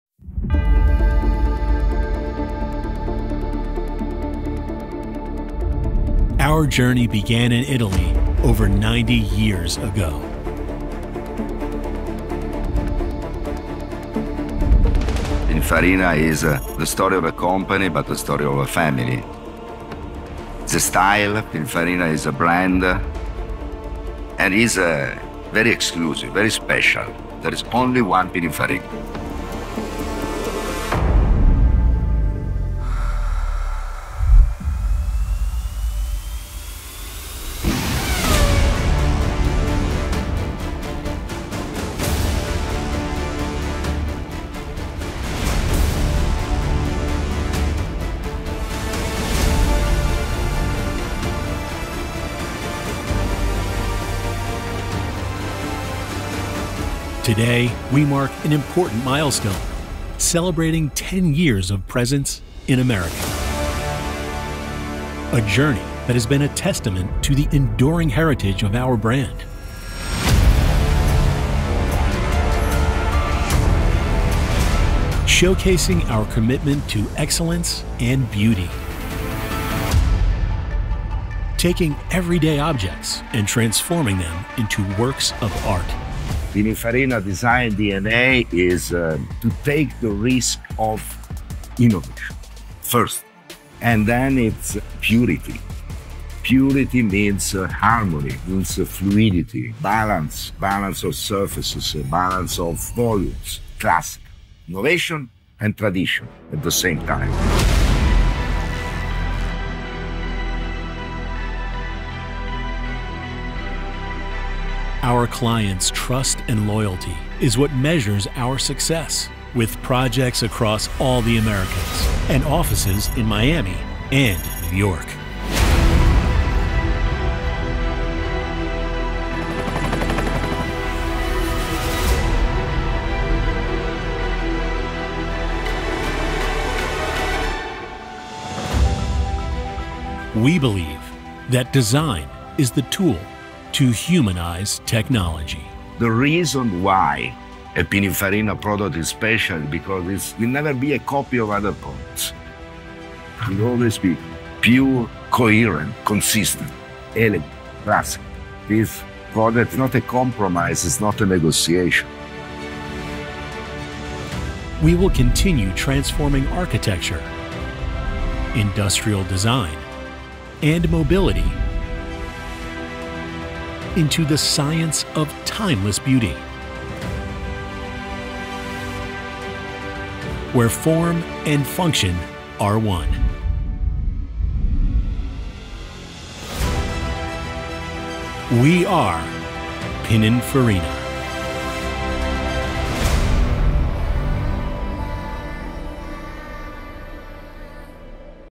Celebrating 10 Years of Pininfarina in America
I am very proud that we were asked to produce the emotional film for the celebrations of 10 years of Pininfarina in America. After more than 90 years, Pininfarina continues to represent an unmistakable "sign" for car enthusiasts but not only; buildings, furniture and everyday objects. Pininfarina... certainly an "Italian Affair".